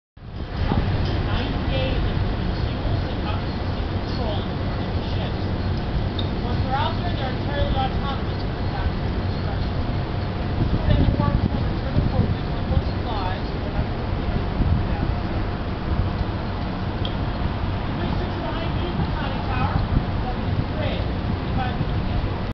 Mr. John Tutorial : Submarine on Lake Michigan, Manitowac Wi.
Submarine on  Lake Michigan (new crew gets the once over on weapons before receiving uniforms)- I was wandering around Manitowac Wi. while waiting on my tanker of 'liquid smoke' to be unloaded.